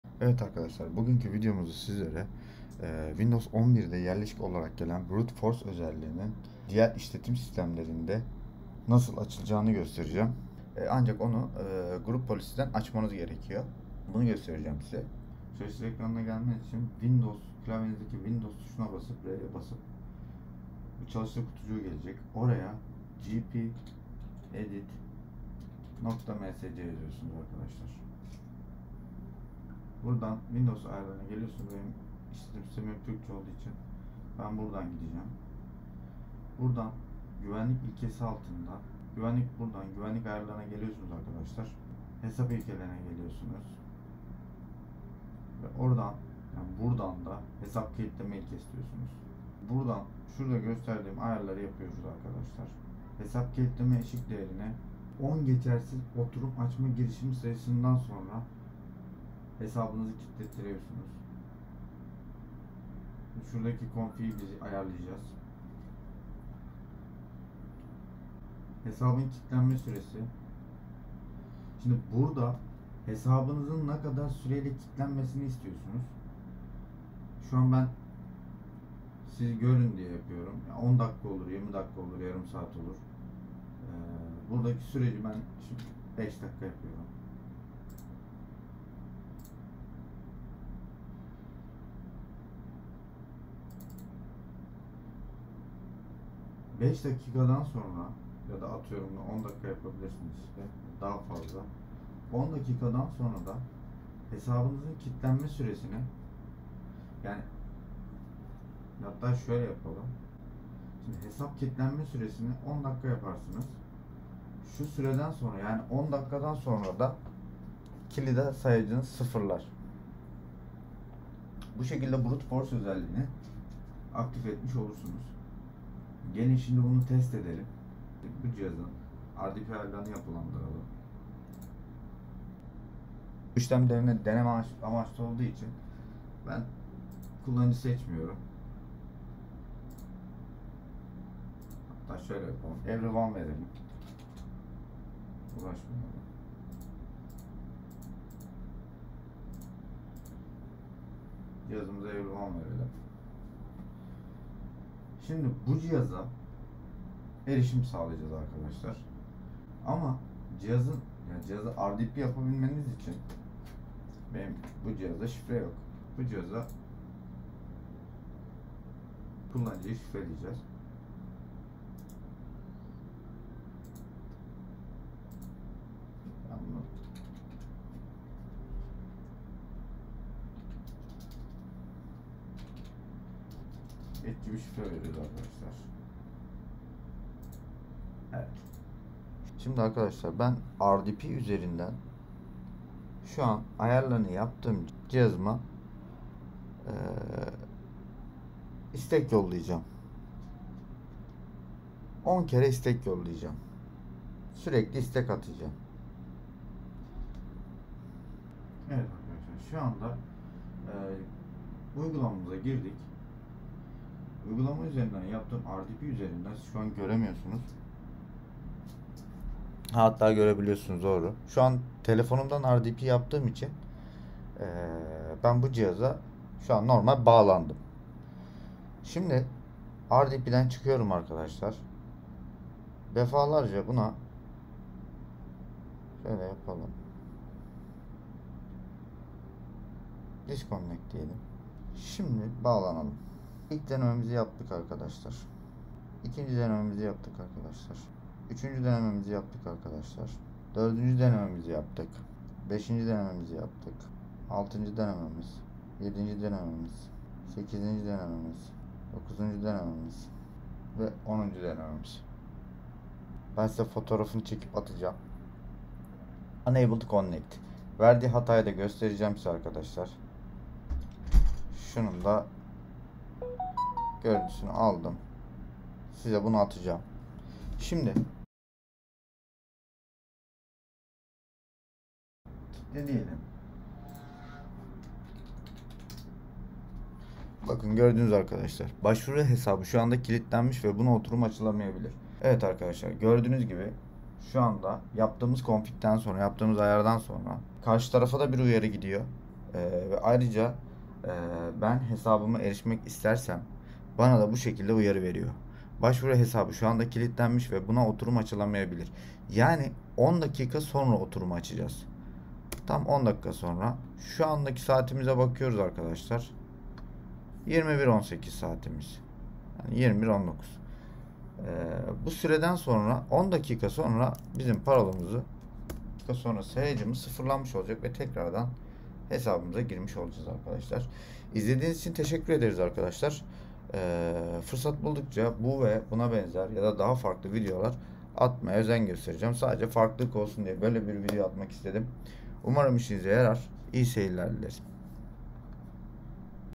Wİndows 11 ile gelen Brute Force özelliğinin açılması ve RDP ayarının yapılarak denenmesi.
Arkadaşlar selam ,

Bu videomuz da sizlere Windows 11 ile varsayılan olarak gelen Brute Force özelliğinin diğer işletim sistemlerinde açılmasını göstermeye çalıştık ,

Umarız işinize yarar,
İyi seyirler.